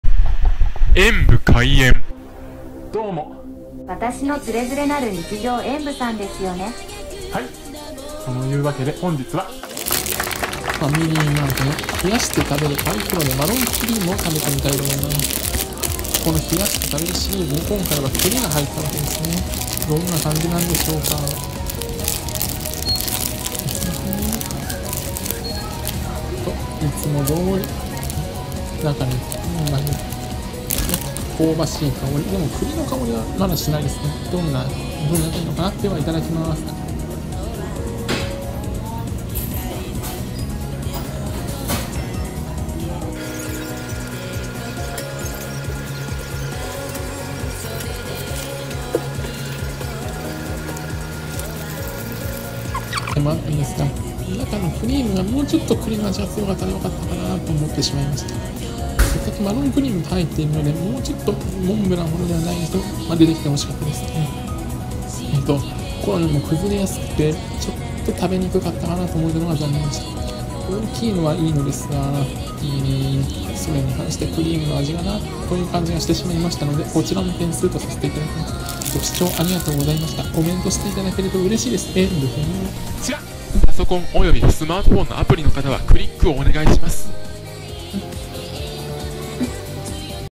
ファミリーマート 冷やして食べるパイコロネを食べてみました。栗ームが本当にパイと合う！！
ファミリーマート　冷やして食べるパイコロネを食べてみました。相変わらず冷やす意味は……ですが、今回はクリームが美味しい！！
贈り物は此方まで↓
〒153-0042